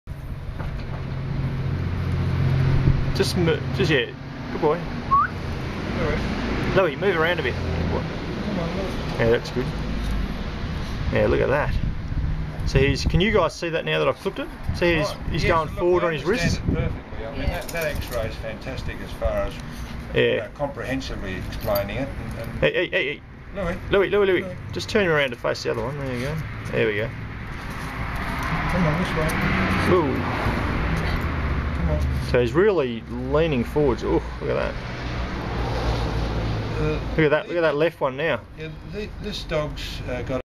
antebrachial deformity shaved.avi
Video footage before and after a major surgery